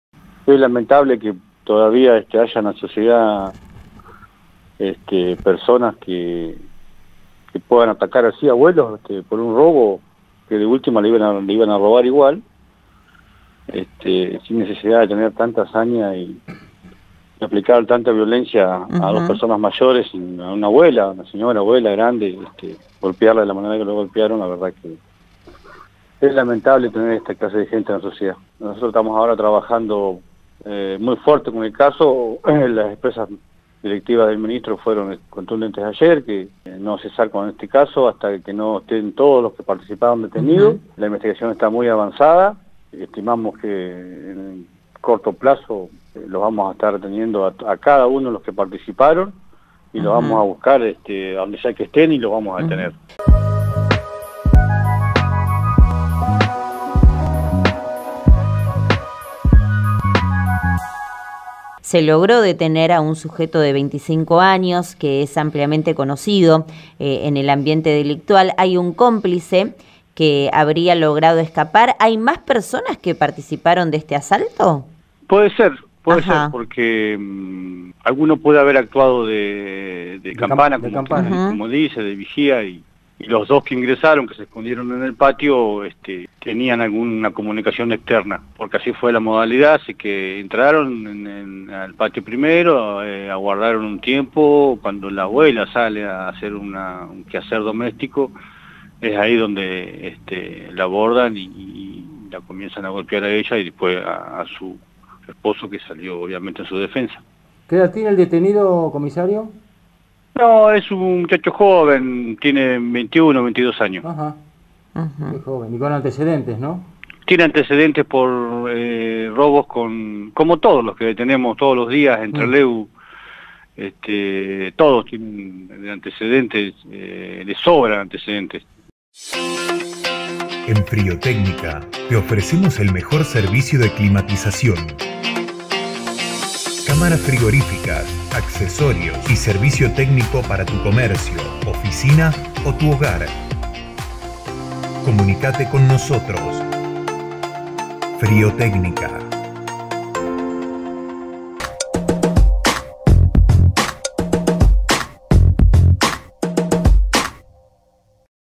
Brutal asalto a una pareja de ancianos en Trelew: un detenido y un prófugo
Sucedió durante el mediodía de ayer, en un domicilio en la calle Gales y Marconi. Las víctimas, de 77 y 79 años, fueron trasladadas al Hospital debido a los golpes recibidos.
📢 Néstor Gómez Ocampo – 2do jefe policía del Chubut